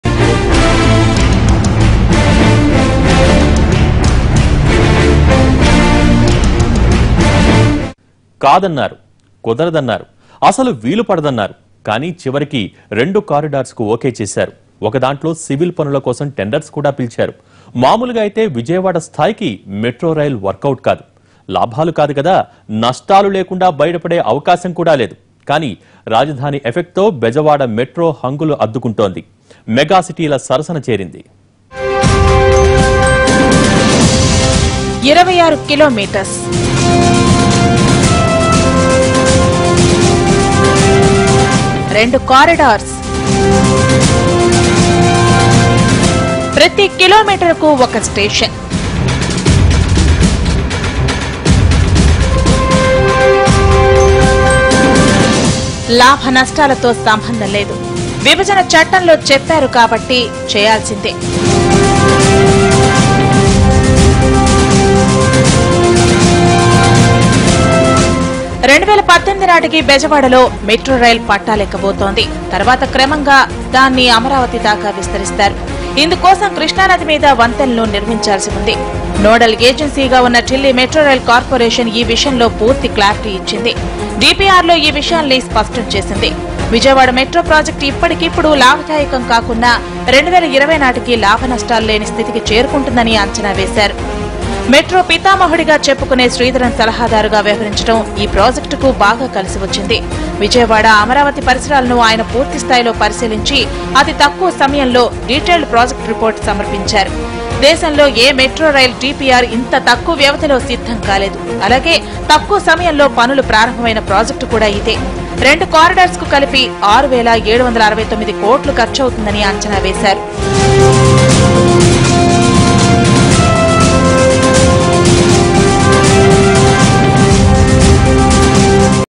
Ground Report on AP Capital Vijayawada | Amaravathi Ankurarpana | Focus | Part 3
NTV Special Grtound Report on AP Capital and the development works has started in Vijayawada by AP CM Chandrababu Naidu to make Vijayawada as a Smart City.